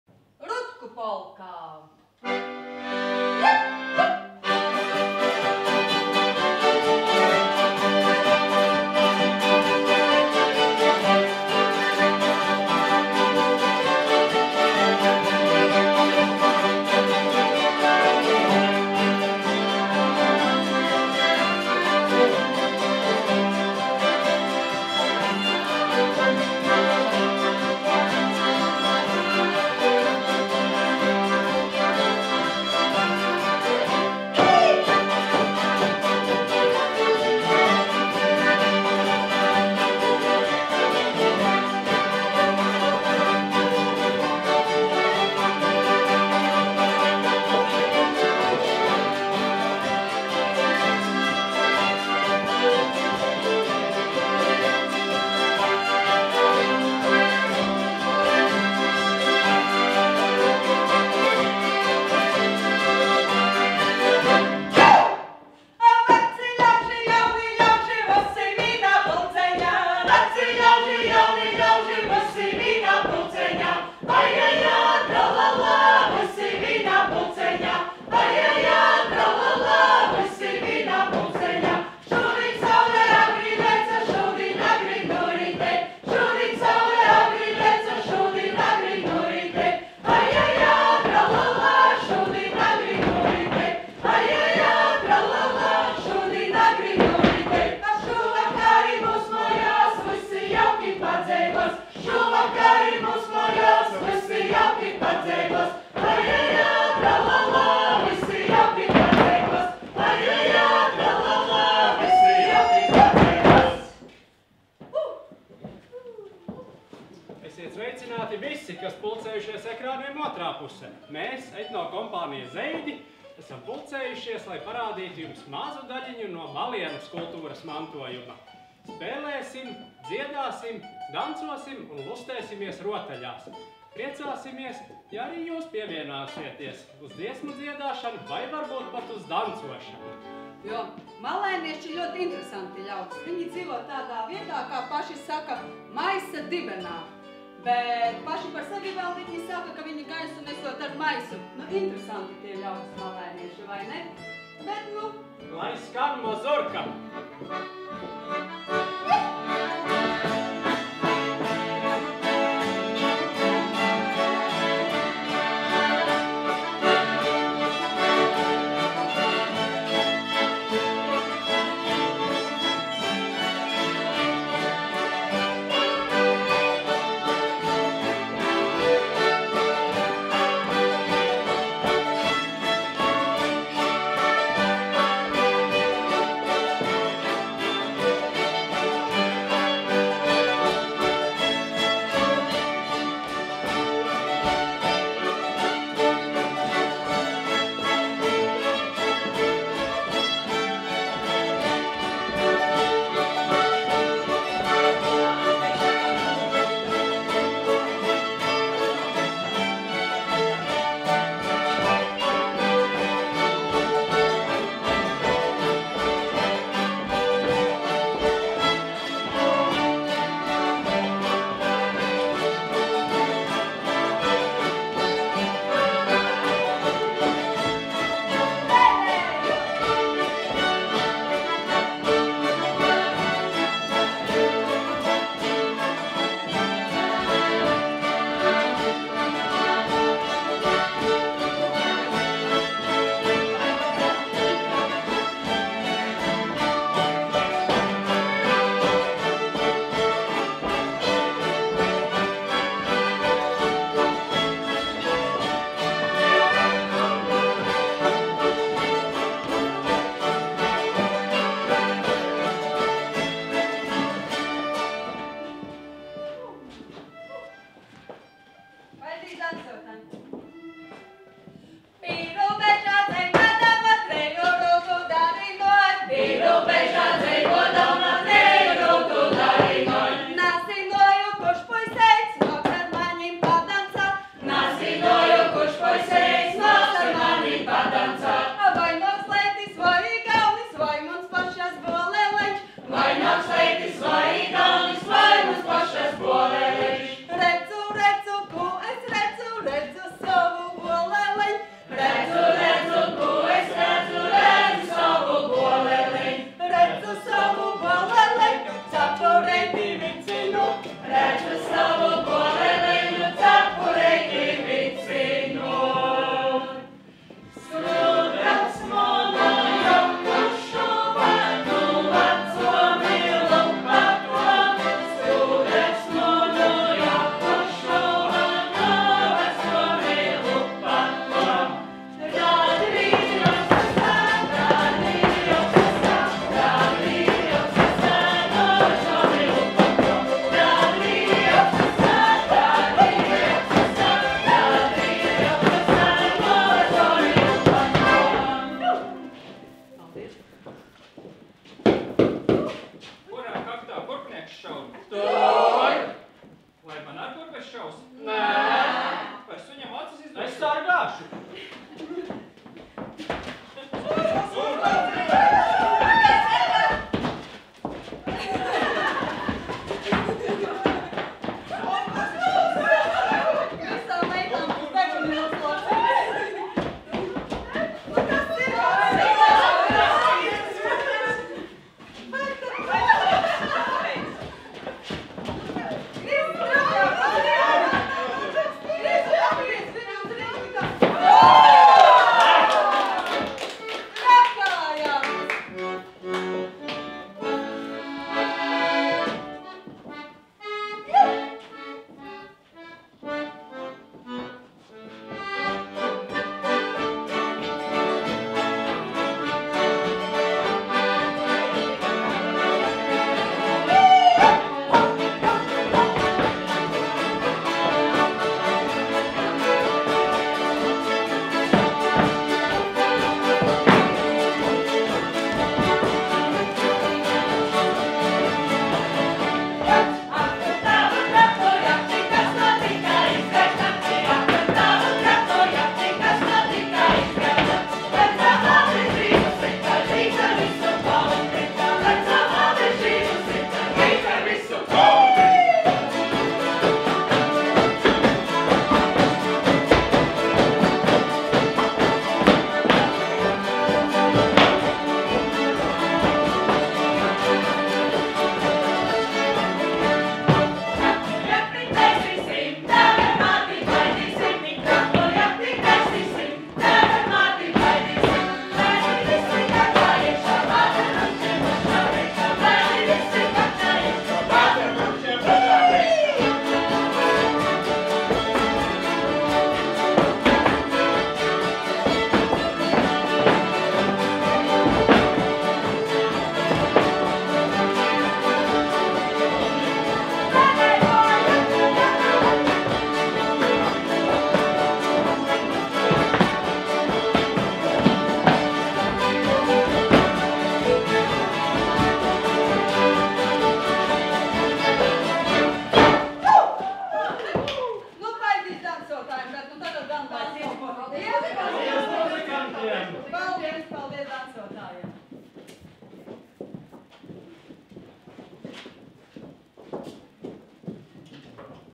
Etno kompānija Zeidi Folkloras festivāla Baltica 2022 skates programma
Etno kompānija Zeidi Balticas 2022 skatē startē ar Malienas kultūras mantojumu - jestrām rotaļām, mūziku un dančiem.